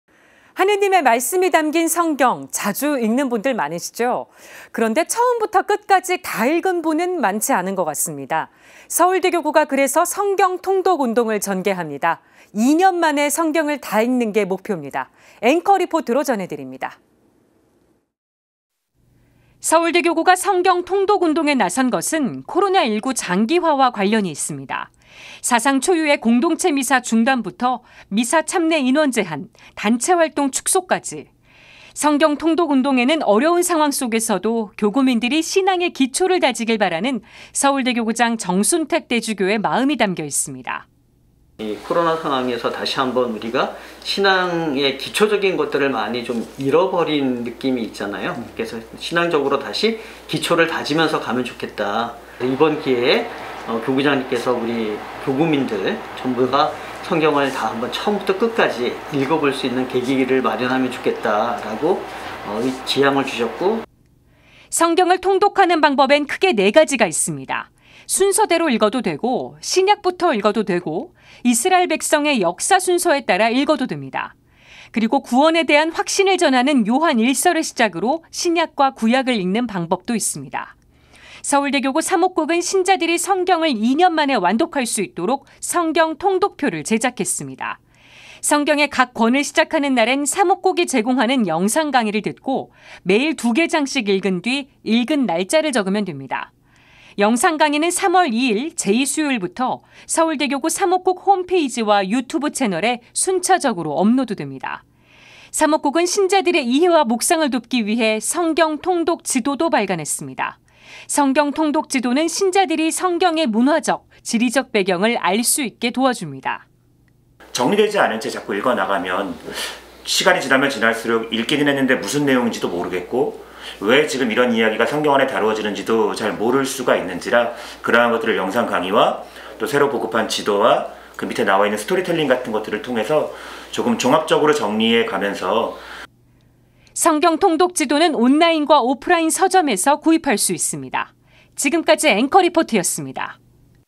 서울대교구 성경통독운동 전개···"성경 끝까지 읽어봅시다"
하느님의 말씀이 담긴 성경,
자주 읽는 분들 많으시죠.

그런데 처음부터 끝까지
다 읽은 분은 많지 않은 것 같습니다.

서울대교구가 그래서
성경통독운동을 전개합니다.

2년 만에 성경을 다 읽는 게 목표입니다.
앵커 리포트로 전해드립니다.

후원문의, 매일미사 지향 신청 : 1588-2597
카카오톡 플러스친구 : cpbc TV